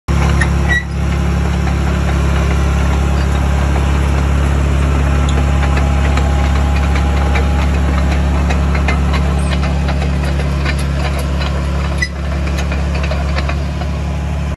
1994 CATERPILLAR D5C III For Sale
Cab, 7,000 Hours, Good Undercarriage, New Idlers being Installed, 6 Way Blade